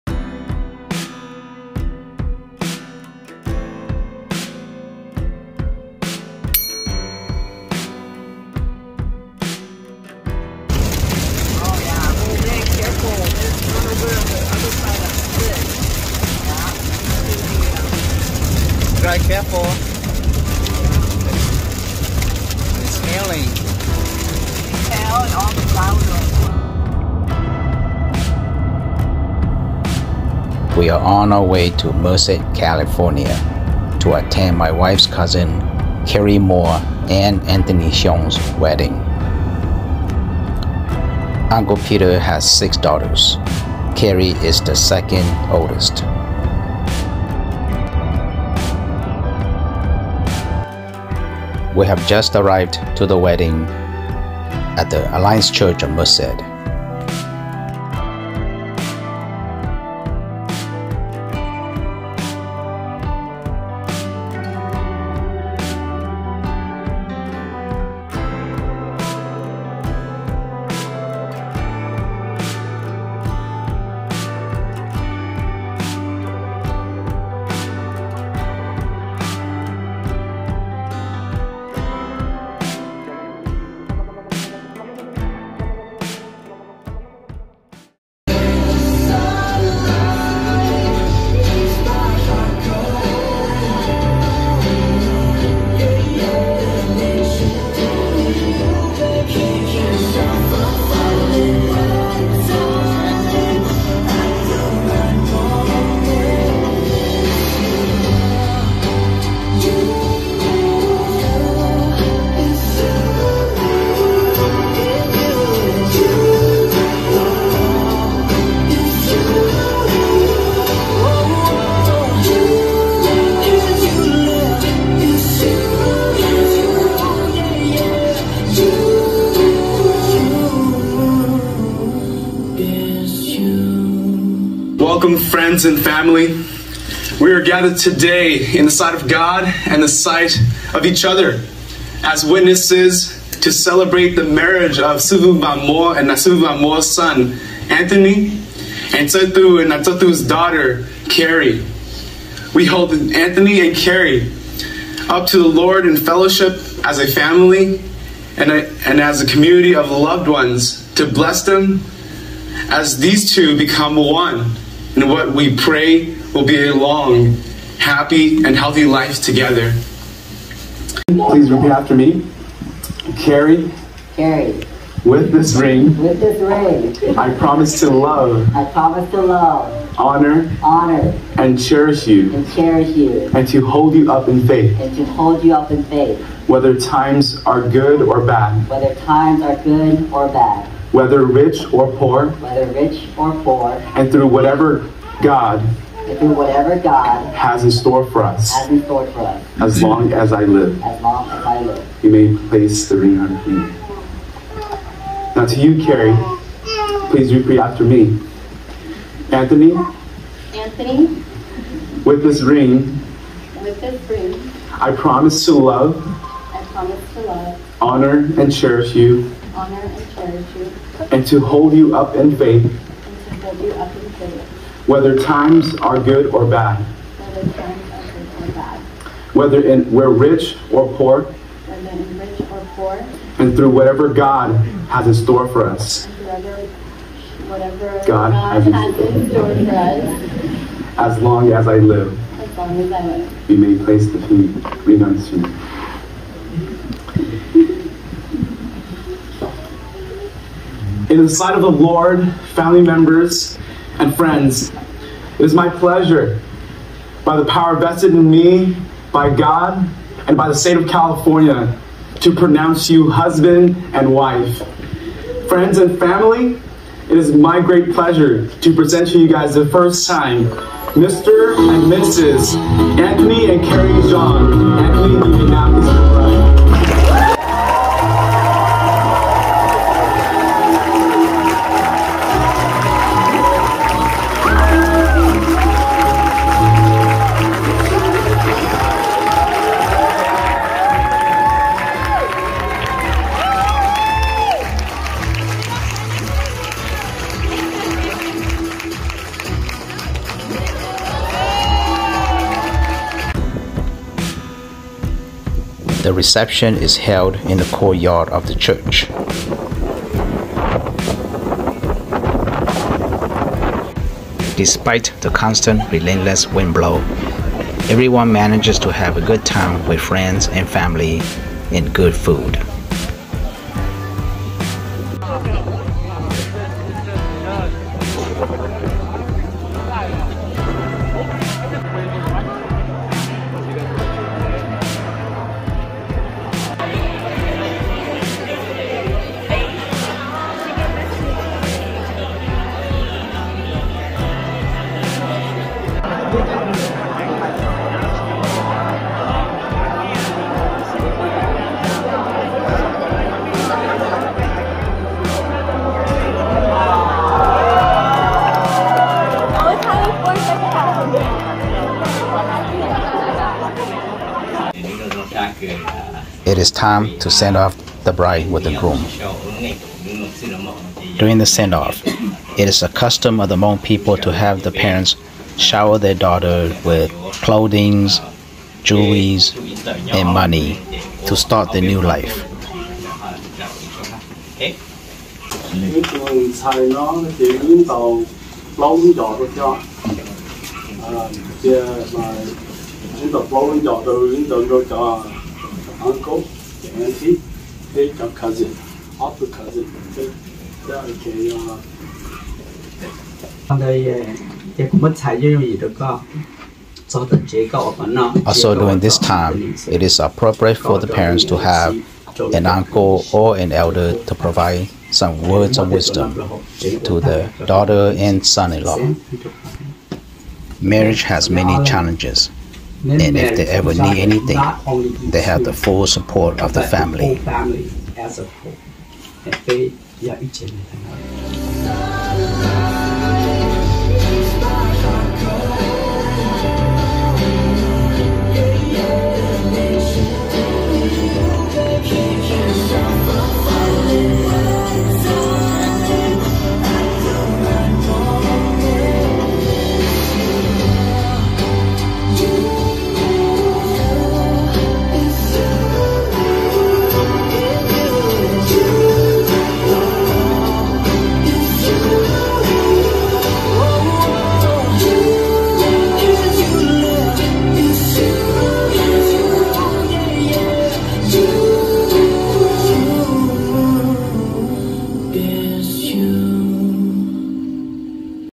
Roadtrip through Washington, Oregon, California Day 4: A Wedding | Moog koom ib rooj tshoob
Our roadtrip from Washington to California to attend Uncle Peter Moua's daughter Karrie Moua and Anthony Xiong's wedding in Merced, California